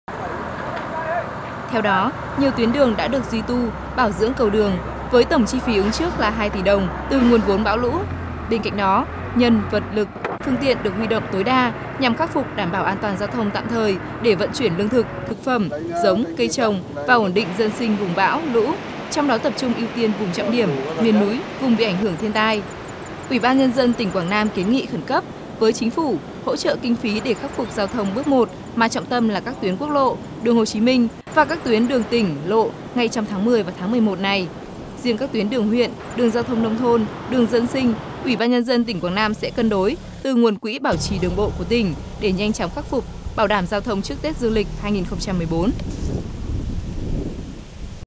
VTC14_Quảng Nam: Cơ bản khắc phục giao thông sau bão
Trên các tuyến quốc lộ và các tuyến tỉnh lộ ở Quảng Nam có hàng trăm điểm sạt lở với khoảng 100.000m3 gây trở ngại lớn cho giao thông. Ước tính tổng thiệt hại lên gần 200 tỷ đồng. Ngay sau khi Bộ GTVT có kiểm tra và chỉ đạo khắc phục, đến nay hầu hết các tuyến giao thông cơ bản được thông tuyến bước 1.